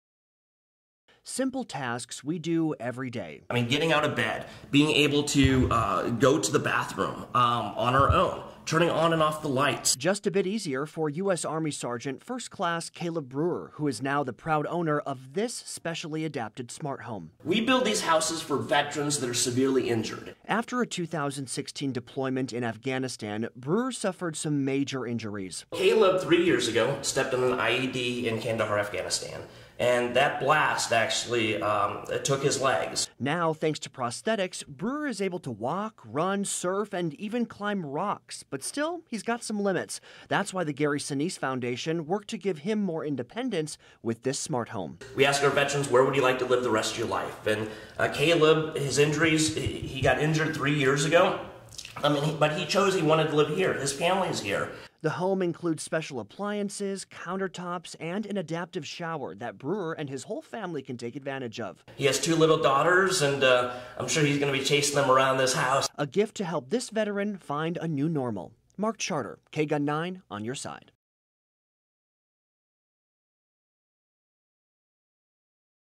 Gary Sinise Foundation provides smart home to U.S. Army Sergeant First Class Caleb Brewer
Simple tasks we do every day, now just a bit easier for U.S. Army Sergeant First Class Caleb Brewer, who is now the proud owner of the home.